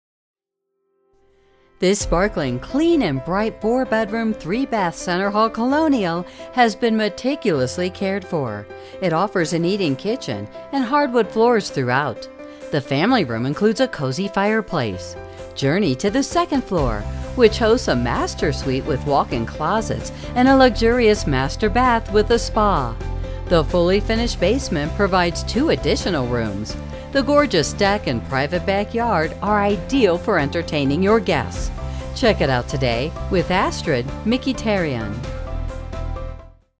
111 Church Street Alpine, NJ | ColdwellBankerHomes.com
111 Church Street, Alpine, NJ

This sparkling clean and bright center hall colonial has been meticulously cared for by its present owners. The family room offers a cozy fireplace for your winter holidays. It exits to an extra large gorgeous deck and a private backyard surrounded by matured trees. The living room includes an artis